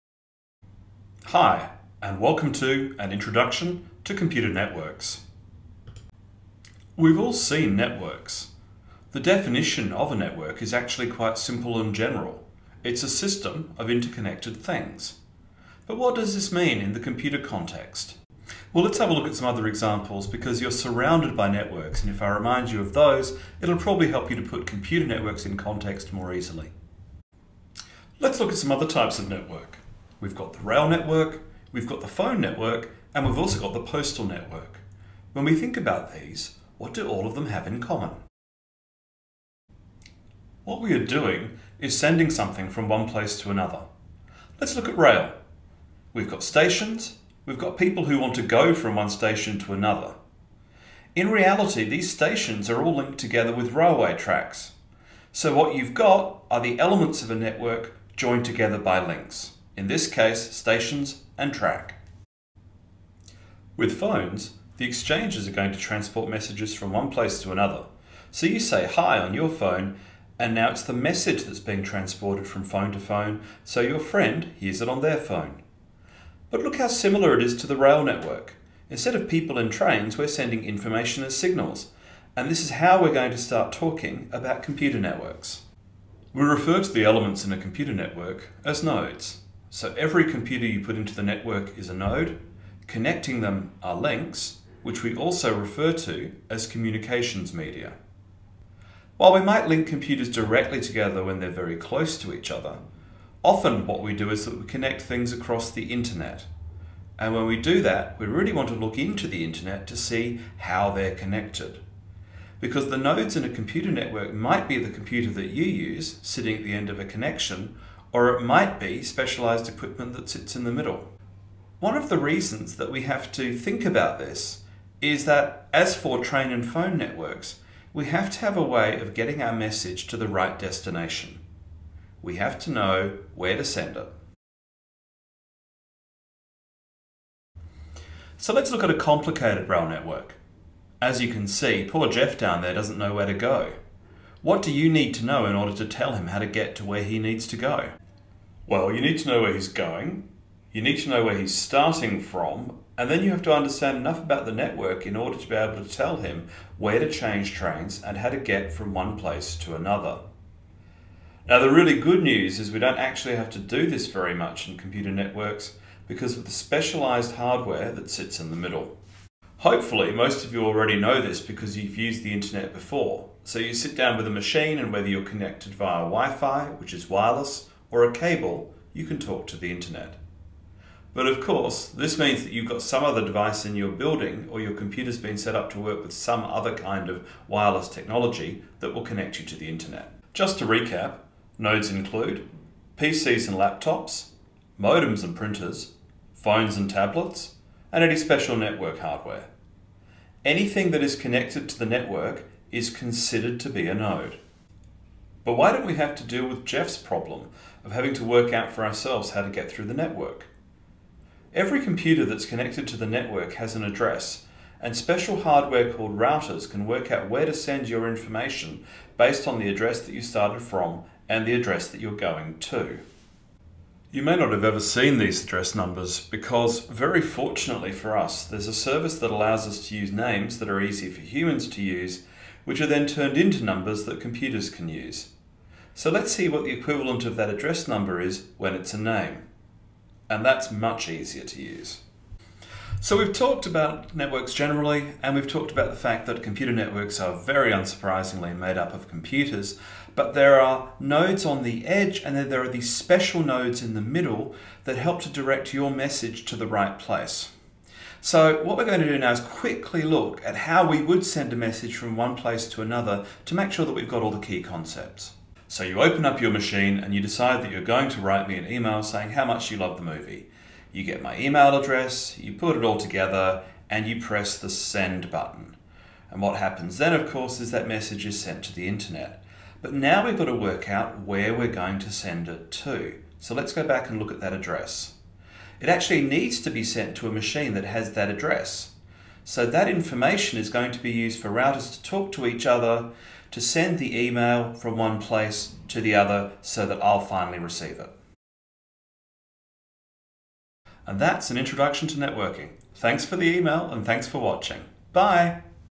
An Introduction to Computer Networking
This video introduces computer networks, using real examples of other types of networks, and shows how data is sent through the network.